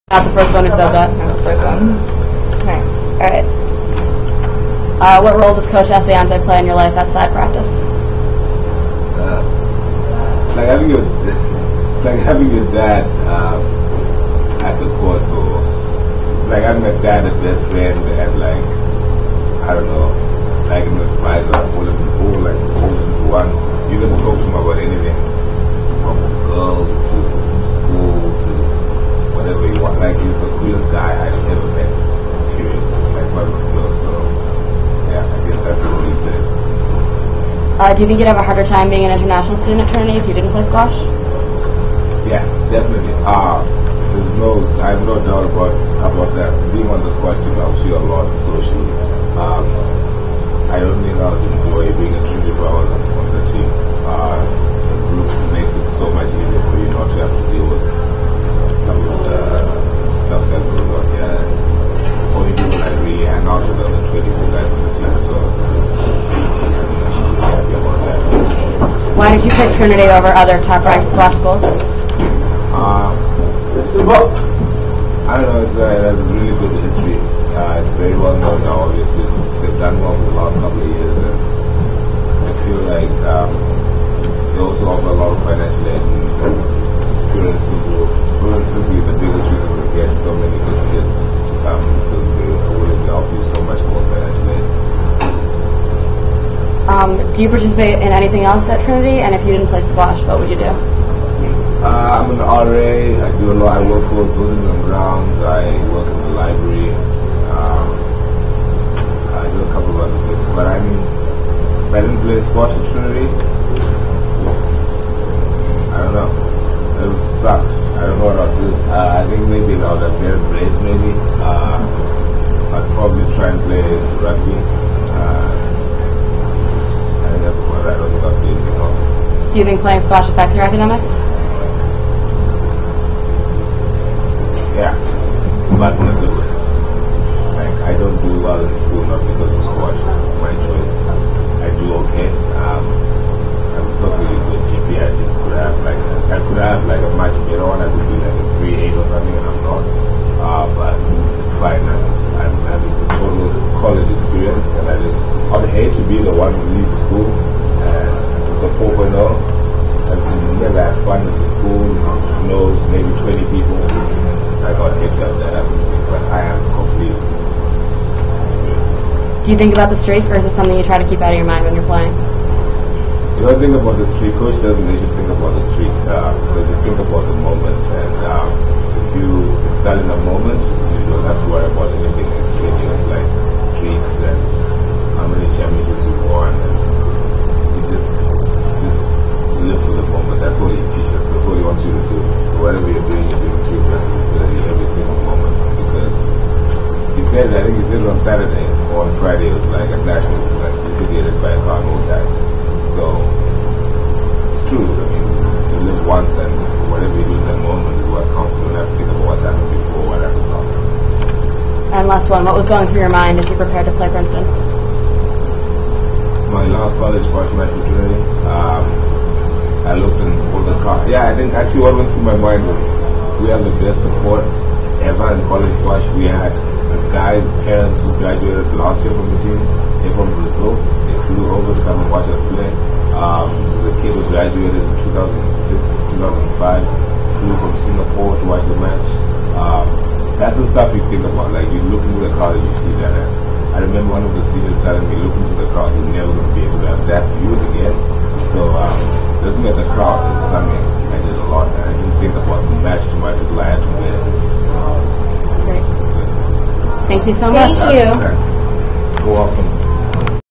Trinity Tripod interviews men's squash team: Simba Muhwati
In recognition of the Trinity Bantams amazing 10th consecutive CSA National Championship (and 183rd straight victory), the Tripod conducted interviews of several members of the squad.

*Apologies for the poor sound quality. We're looking into better sound equipment.